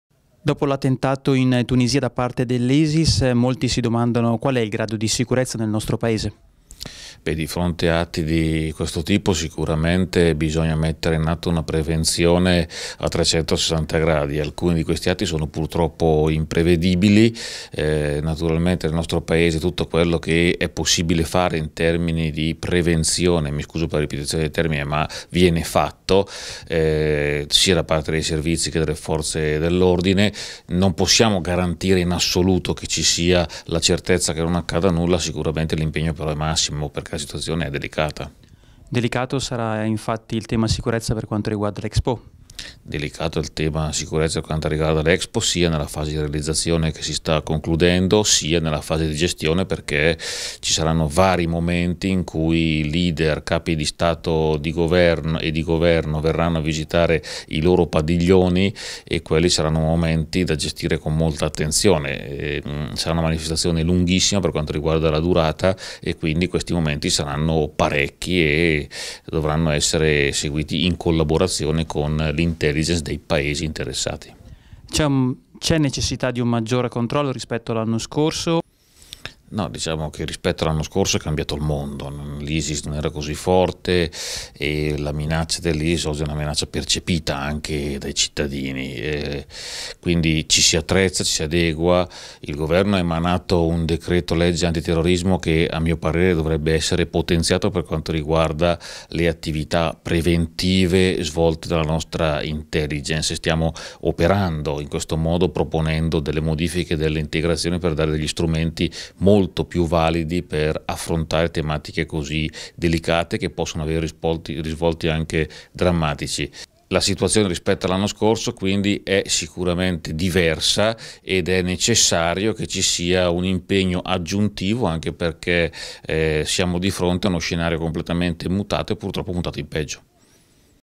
Terrorismo - Stucchi, la situazione è diversa occorre più attenzione
Stucchi: occorre mettere in atto una prevenzione a 360 gradi. Il decreto anti-terrorismo del governo non basta, ma va potenziato.